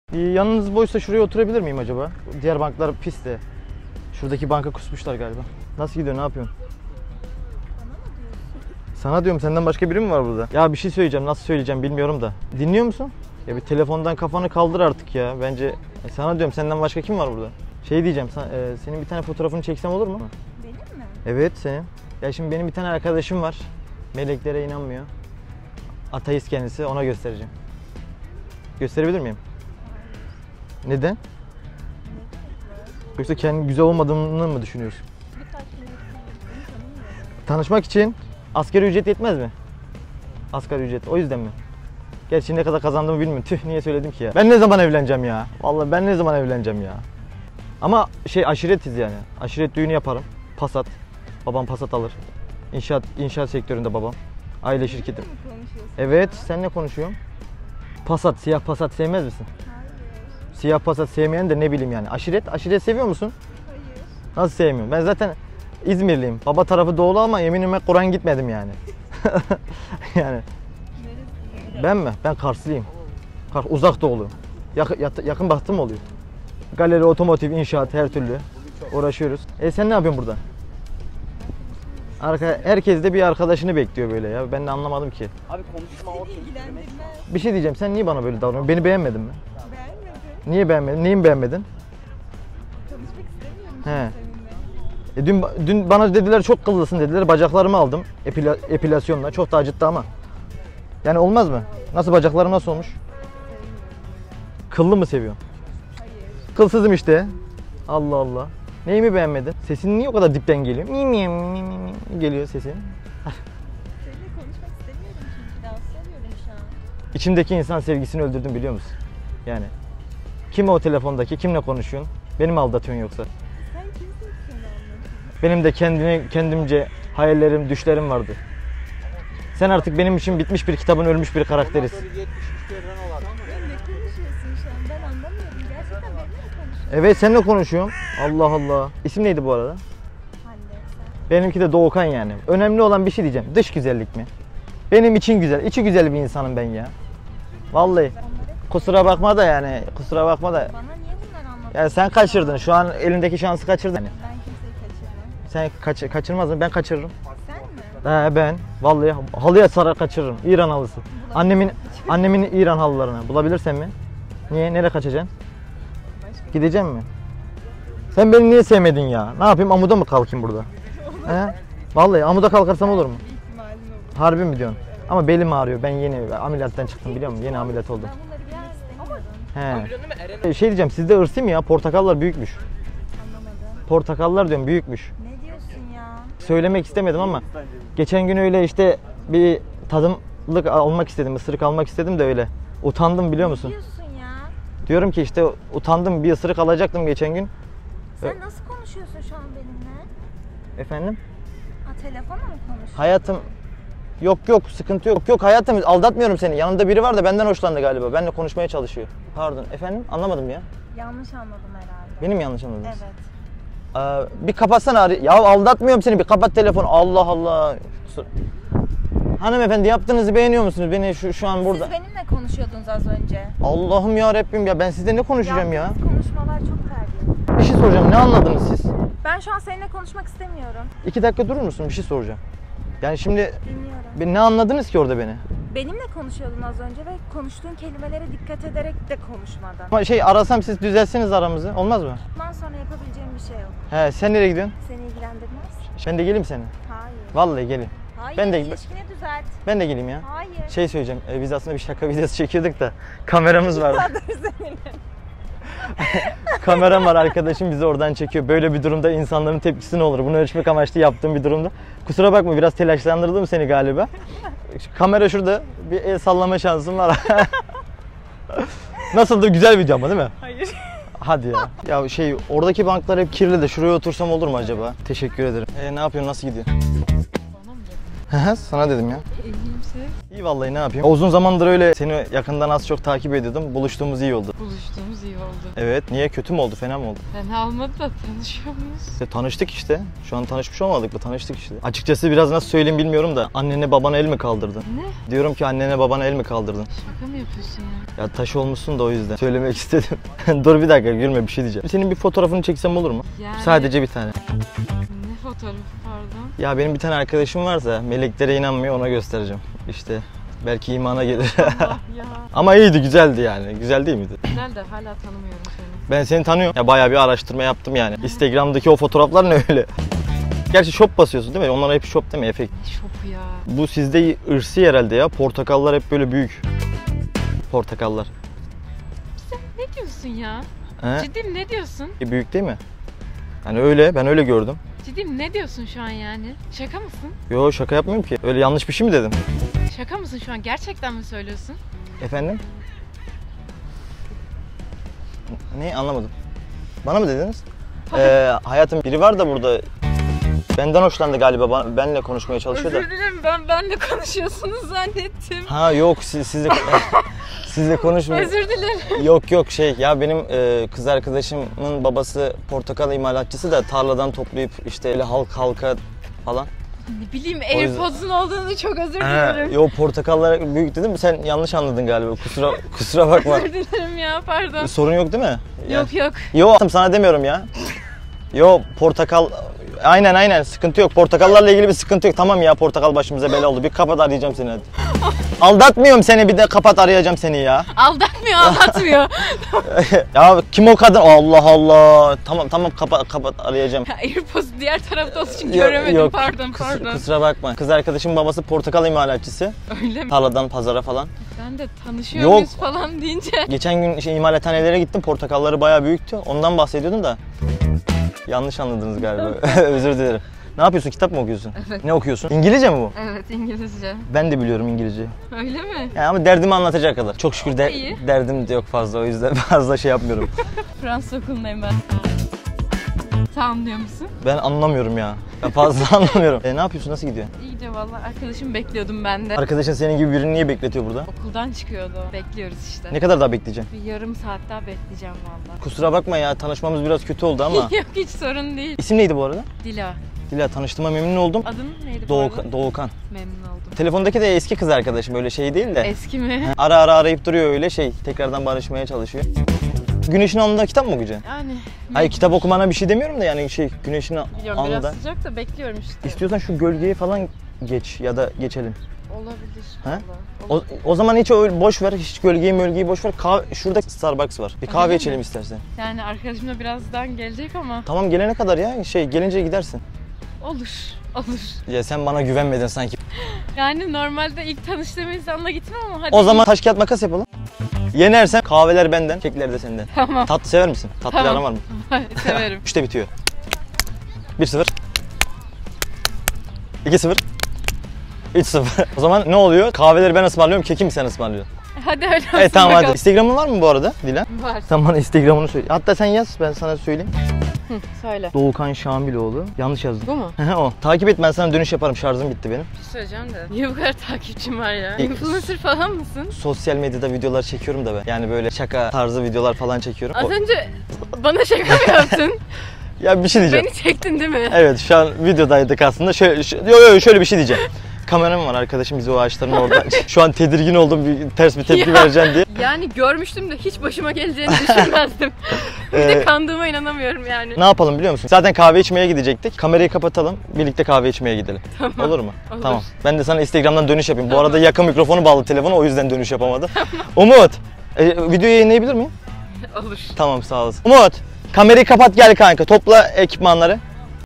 KAVUNLAR BÜYÜKMÜŞ - ABSÜRT KOMİK BLUETOOTH KULAKLIK ŞAKASI
KAVUNLAR BÜYÜKMÜŞ - ABSÜRT KOMİK BLUETOOTH KULAKLIK ŞAKASI Absürt Komik Bluetooth Kulaklık Şakası Kızlara fark ettirmeden Bluetooth kulaklıkla konuştum . Kız her şeyi üzerine alındı . Öyle bir şey dedi ki çok şaşıracaksınız . Umarım videoyu beğenirsiniz .
İyi Seyirler ...